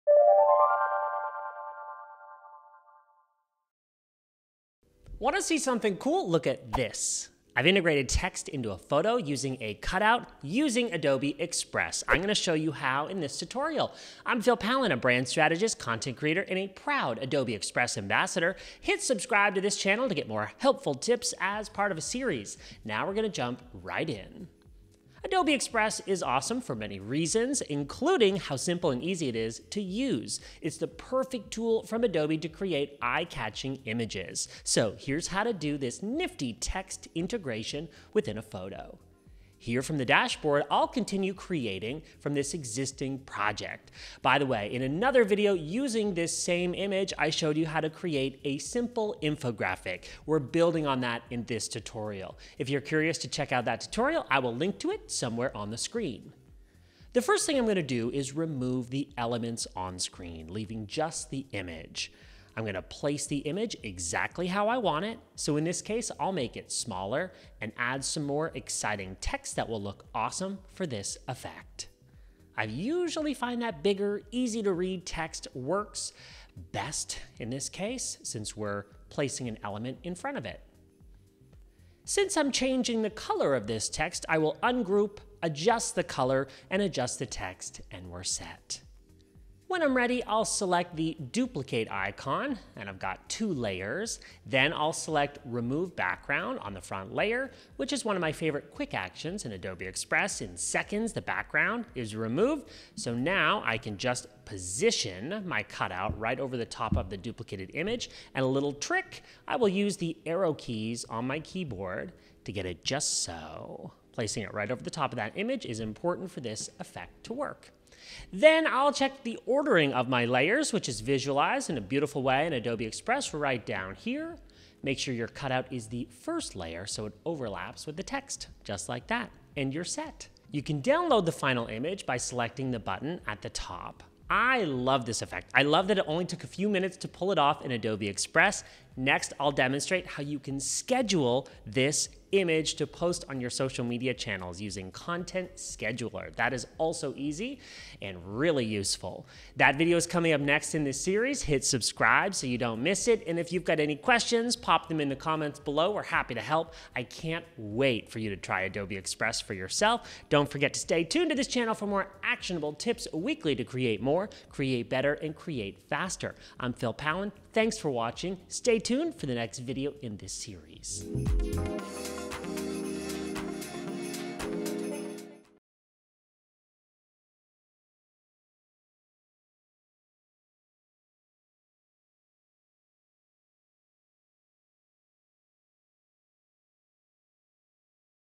How to Use A Cutout to Integrate Text into Photo | Adobe Express
Learn how to easily place text behind any photo or cutout for your small business, social media posts, content creations, and more! Phil Pallen, Adobe Express Ambassador and brand strategist will show you how to transform a template.